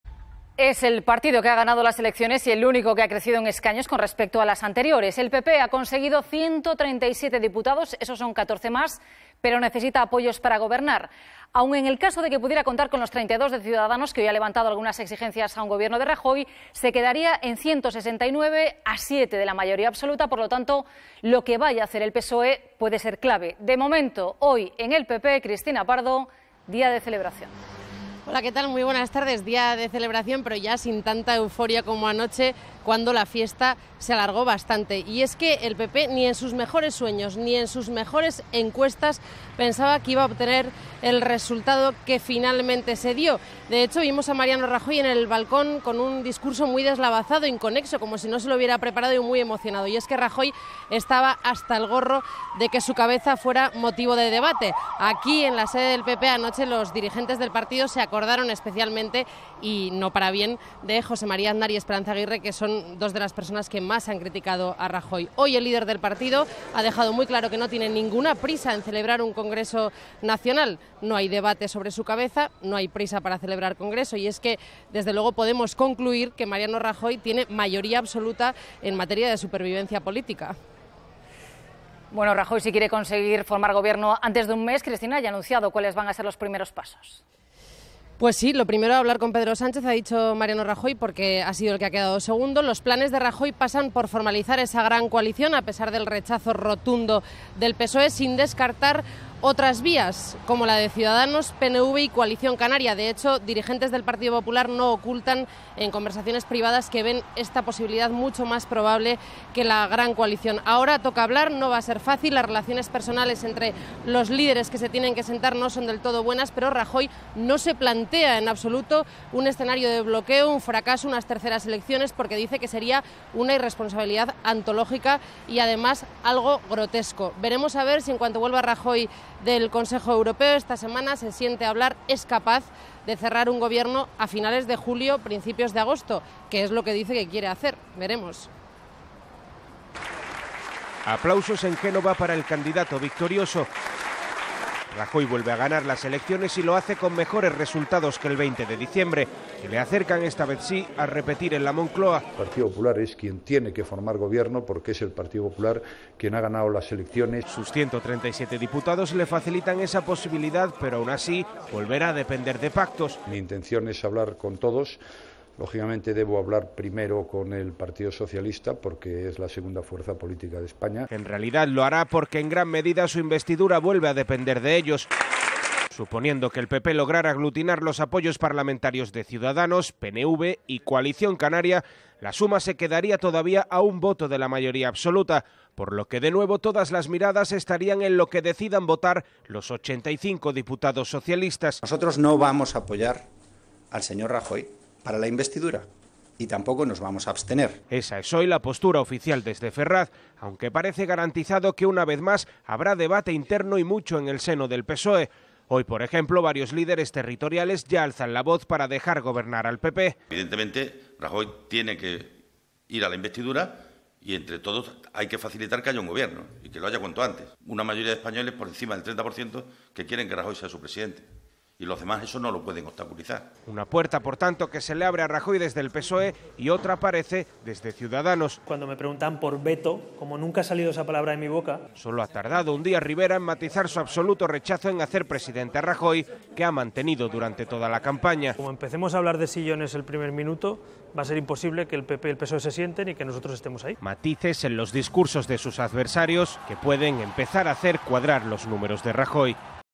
Rajoy apuesta por la gran coalición con el PSOE pero no descarta gobernar en minoría
El PP ha conseguido 137 diputados, pero necesita apoyos para gobernar. Aun en el caso de que pudiera contar con los 32 de Ciudadanos, se quedaría a 7 de la mayoría absoluta. Lo que vaya a hacer el PSOE puede ser clave.